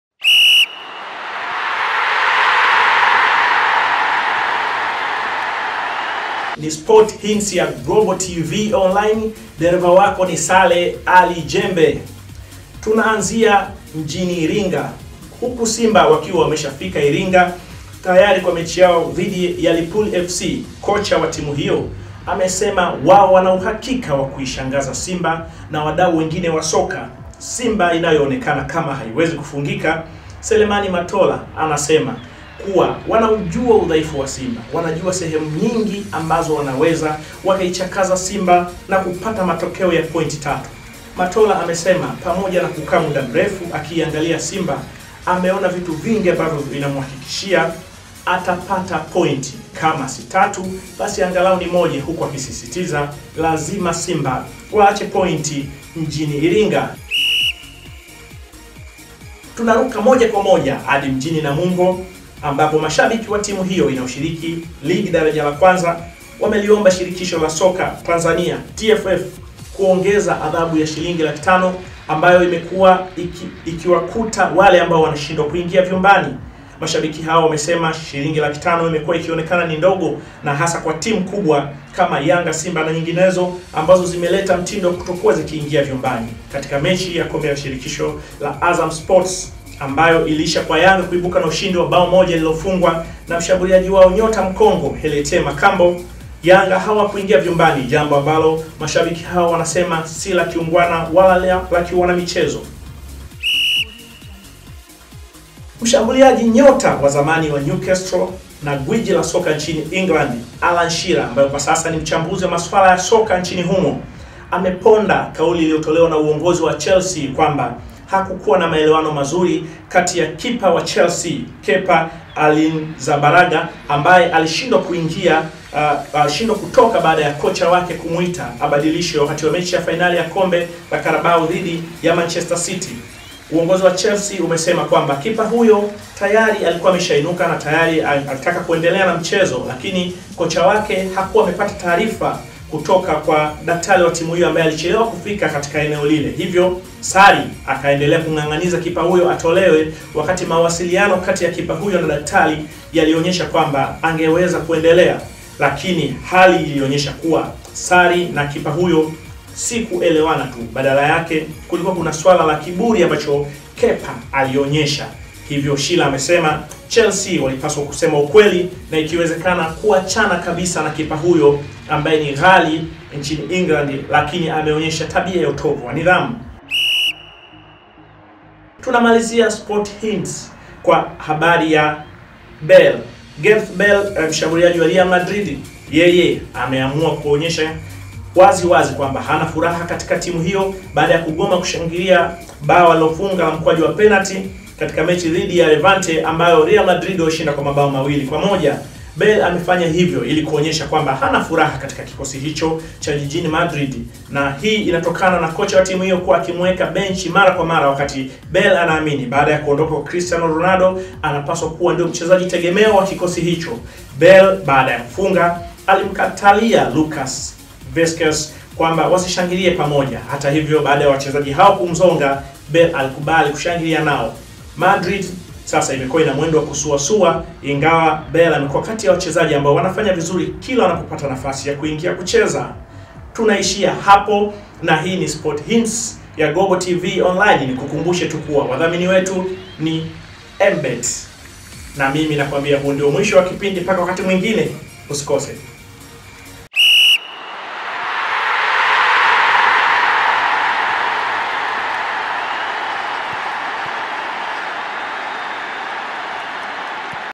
SPOTI HINTS: MATOLA, TUNACHUKUA POINTI 3 KWA SIMBA
LICHA ya Simba kushinda mechi zake zote za hivi karibuni kwa kuanza na Mwandui 3-0, akafuata Mwarabu 1-0, African Lyon 3-0, Yanga 1-0 na Azam FC 3-1, kocha wa Lipuli, Selemani Matola amesema atawafunga Simba.

Simba itacheza kesho na Lipuli mchezo wa ligi ikiwa ni kiporo kwenye Uwanja wa Samora huku rekodi zikionyesha kwamba tangu msimu wa 2017/2018 Lipuli ilipopanda daraja, hakuna hata timu iliyoonja ushindi kwa mwenzake.

Matola alisema amewatazama wapinzani wake kwa ukaribu na kujua udhaifu wao, hali ambayo imemfanya aamini kwamba kesho atavunja rekodi iliyowekwa kwa timu hizi mbili.

Katika michezo hiyo mitatu ambayo walicheza nao msimu wa 2017/18 wakiwa nyumbani Simba walilazimisha sare ya kufungana bao 1-1 kwenye Uwanja wa Samora, mchezo wa marudiano Lipuli walilazimisha sare ya bao 1-1 Uwanja wa Taifa.

Katika mchezo wa mzunguko wa kwanza msimu huu, zilitoka suluhu Uwanja wa Taifa.